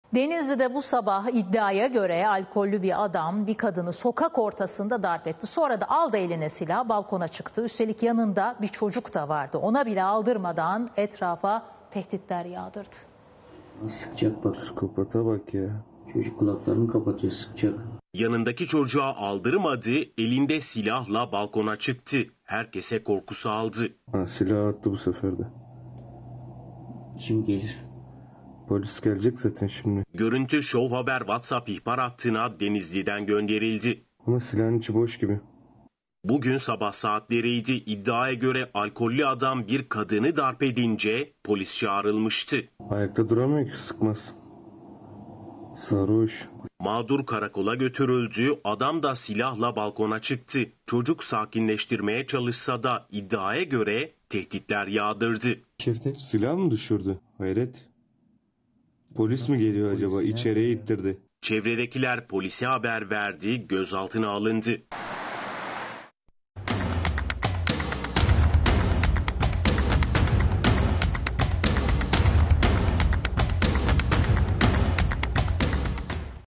Elinde silahla tehdit yağdırdı!
DENİZLİ'DE BU SABAH İDDİAYA GÖRE ALKOLLÜ BİR ADAM BİR KADINI SOKAK ORTASINDA DARBETTİ. SONRA DA ALDI ELİNE SİLAHI BALKONA ÇIKTI. ÜSTELİK YANINDA BİR ÇOCUK DA VARDI. ONA BİLE ALDIRMADAN ETRAFA TEHDİTLER YAĞDIRDI.